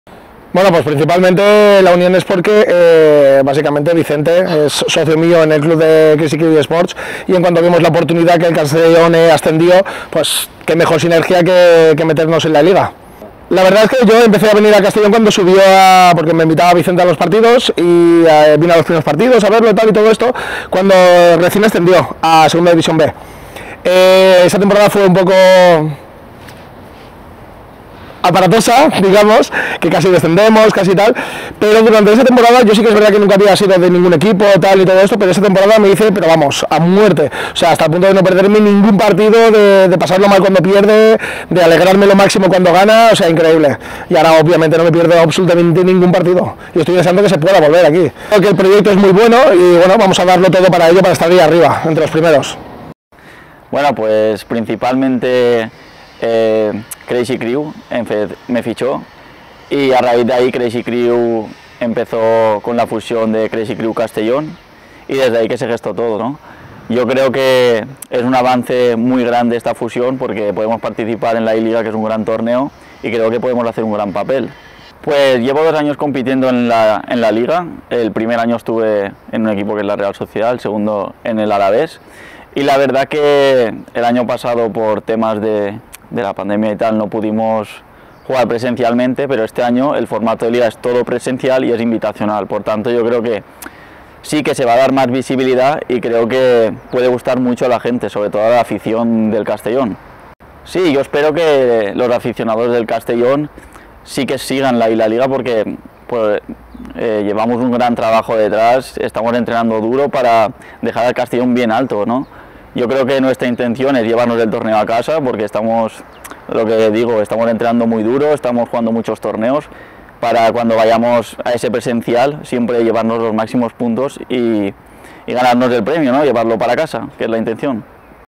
DoctorePoLLo y Luiswdg hablan sobre la unión del CD Castellón y la Crazy Crew
Las dos entidades se unen para competir en eLaLiga Santander